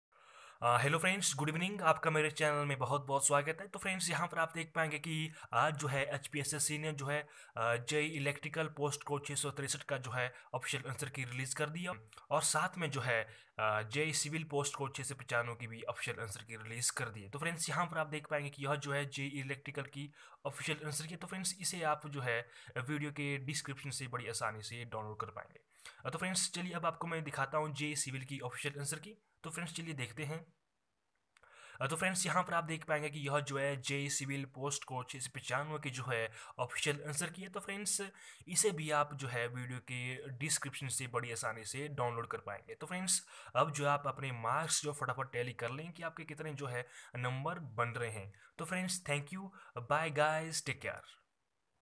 HP JE Electrical /JE Civil Official Anskey || Exam Held on 18 Nov. || Post Code 663/695
Hi guys..
Hpssc/hpsssb released official anskey of je civil post code 695 & je electrical post code 663..
Thanku..
                     *successPOINT 4us*